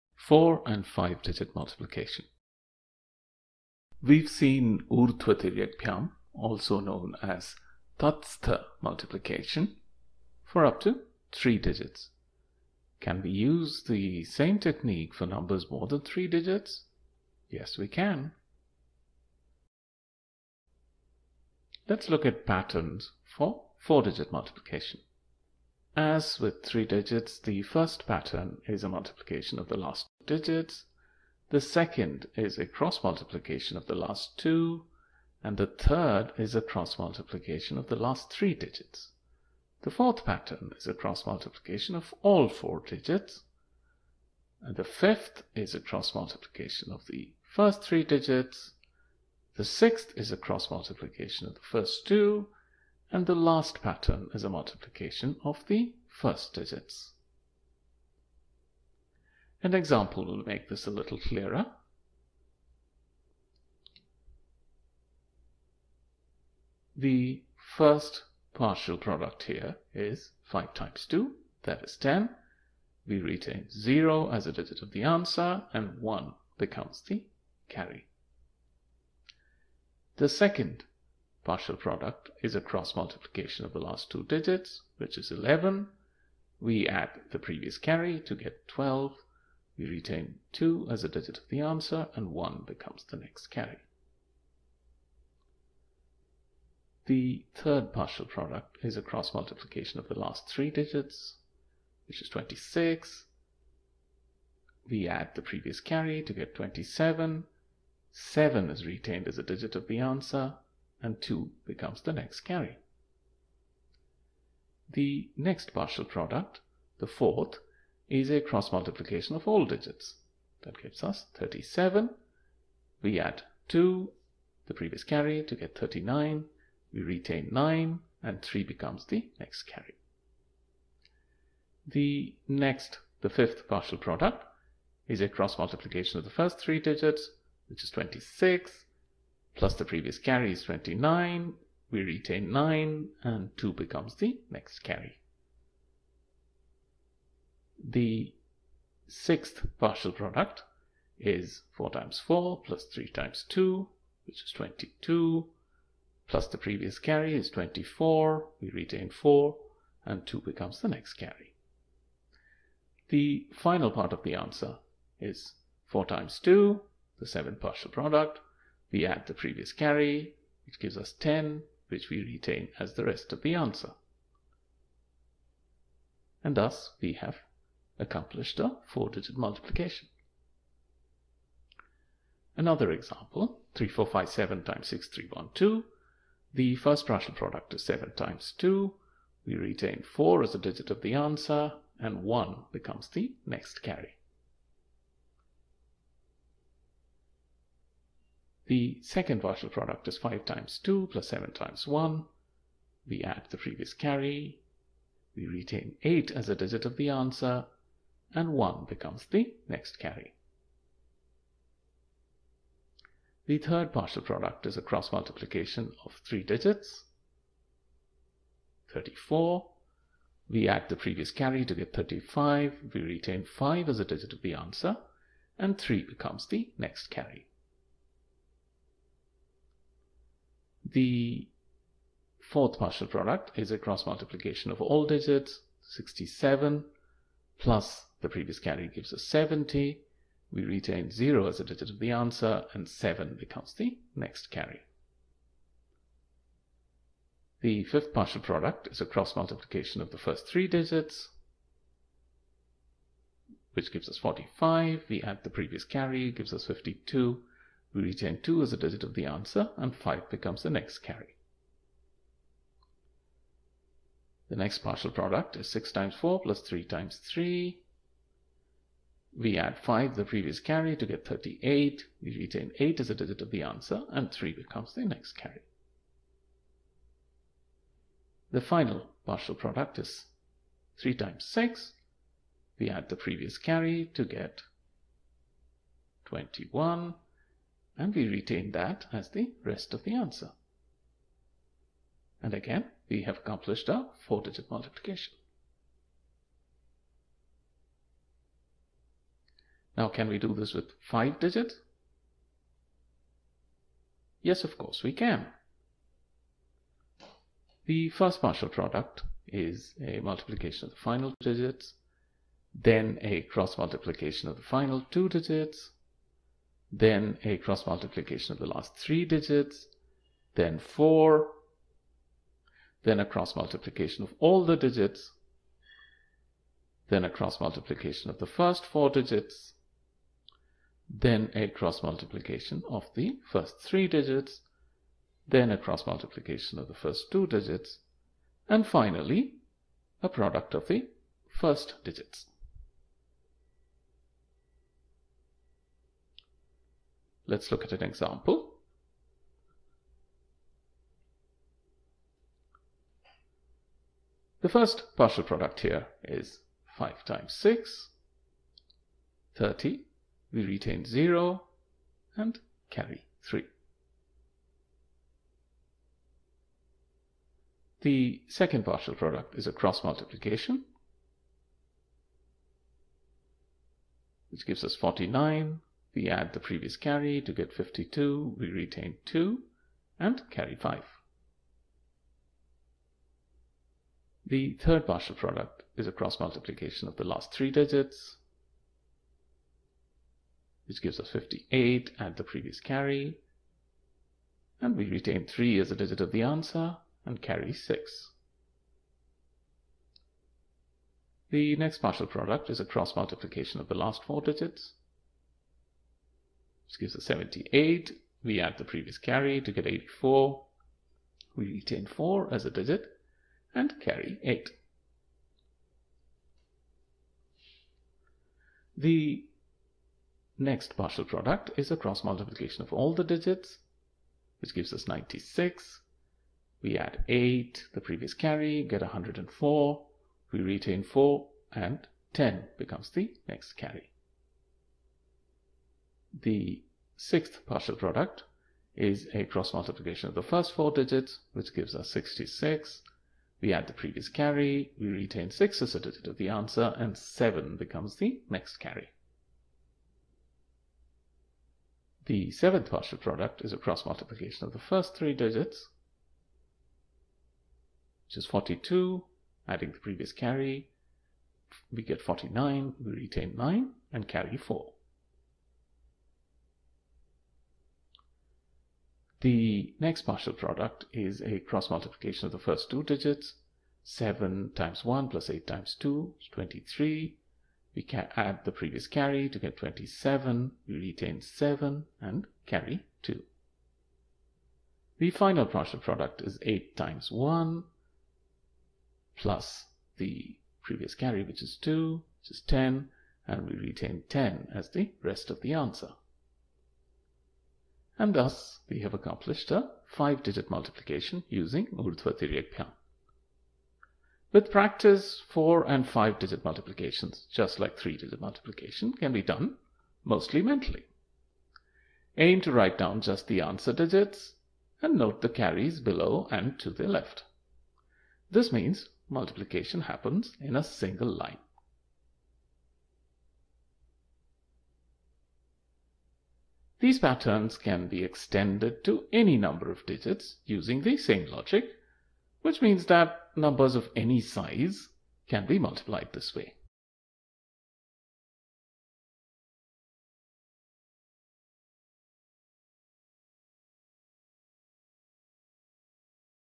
11 | Multiplication of 4 and 5 digit Numbers | Urdhvatiryagbhyam | Vedic Mathematics
Like 2 and 3 digit numbers, we can use the ऊर्ध्वतिर्यग्भ्याम्  sutra to multiply 4 and 5 digit numbers as well. Watch this video to see how 4 and 5 digit multiplication patterns work.

With practice, these can be done mentally as well. At each step, calculate one partial product, add the previous carry, and write down one digit of the answer, and the next carry below and to the left.

Exercises:
1. 2354 * 6934
2. 8793 * 3456
3. 56749 * 49832

Remember to check your answers using the digitsum (samuccaya) method.